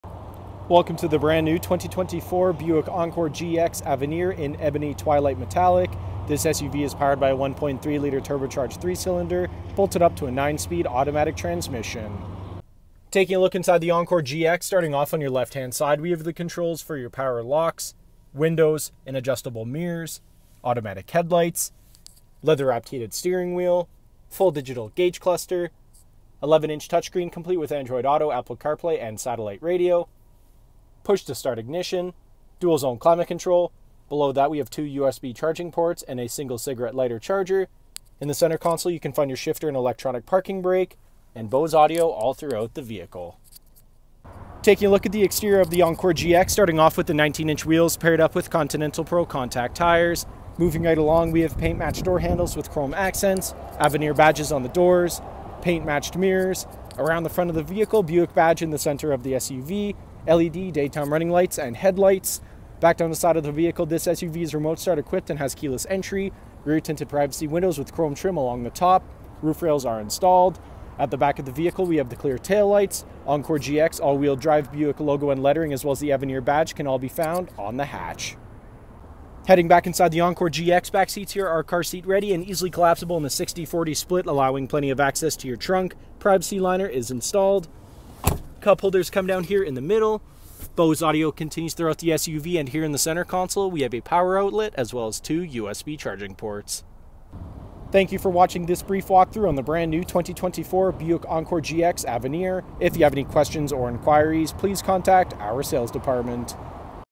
Black 2024 Buick Encore GX Avenir Review Calgary AB - Wolfe Calgary | 4A066911
Introducing the luxurious 2024 Buick Encore GX Avenir, a premium compact SUV that redefines sophistication and comfort. Dressed in the captivating Ebony Twilight Metallic exterior and featuring an Ebony interior with Ebony accents, this vehicle radiates elegance and refinement.

Beneath the hood lies a potent 1.3L Turbocharged 3-cylinder engine paired with a smooth-shifting 9-speed automatic transmission. This powertrain combination ensures responsive performance and excellent fuel efficiency, making every drive a pleasure.

Step inside the cabin, and you'll be greeted by a sumptuous interior equipped with an array of premium features. Sink into the plush heated seats and grip the heated steering wheel, perfect for staying cozy during chilly weather. The focal point of the interior is the expansive 11-inch touchscreen infotainment system, providing seamless access to navigation, entertainment, and connectivity features. Additionally, the 8-inch driver information center keeps you informed about vital vehicle information with ease. Dual-zone climate control ensures personalized comfort for both the driver and passengers, enhancing the overall driving experience.

On the exterior, the Buick Encore GX Avenir exudes sophistication with its heated mirrors, LED headlights, and tail lights. Adding to its upscale appearance are the stunning 19-inch pearl nickel aluminum wheels, which complement the vehicle's sleek profile.

Safety is paramount in the Buick Encore GX Avenir, and it comes equipped with a comprehensive suite of advanced safety features. Rear park assist, rear cross-traffic alert, lane change alert, lane keep assist, forward collision alert, and following distance indicator work together to provide peace of mind and confidence on every journey.

In summary, the 2024 Buick Encore GX Avenir offers a luxurious driving experience with its premium features, refined interior, and advanced safety technologies. Whether you're navigating city streets or embarking on a road trip, this upscale SUV is designed to elevate every moment behind the wheel.